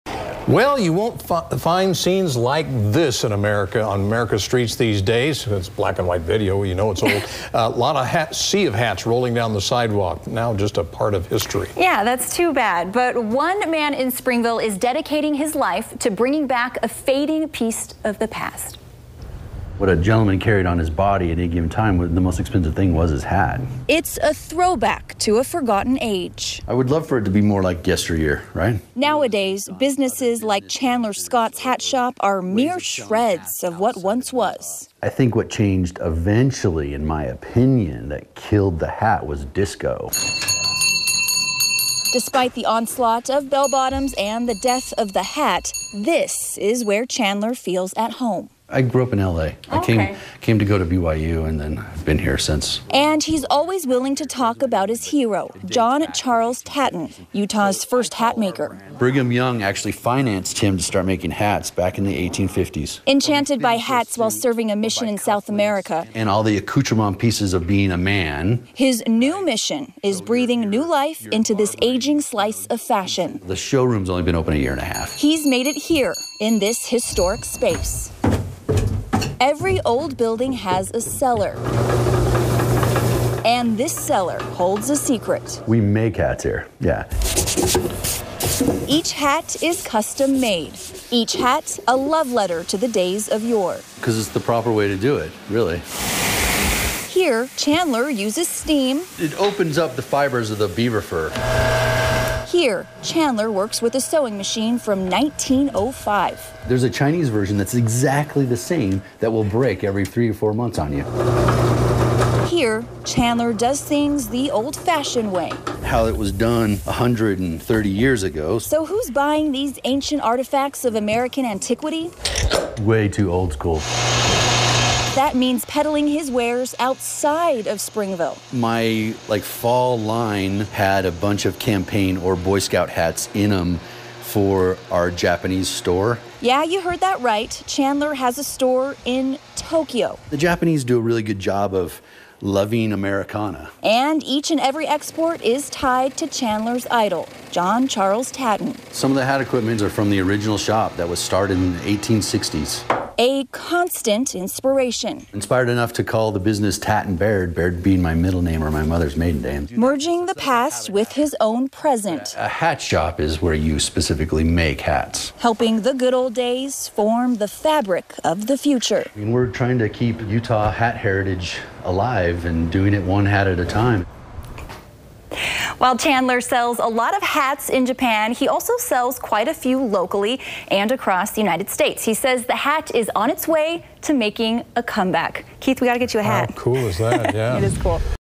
Utah man hearkens back to earlier era with custom-made men's hats
A sea of hats rolling down the sidewalk is now just a part of history. But as Ashley Kewish reports, in Springville, one man is dedicating his life to bringing back a fading piece of the past.
Photojournalist: Ray Boone
Story aired 10pm on 1/1/17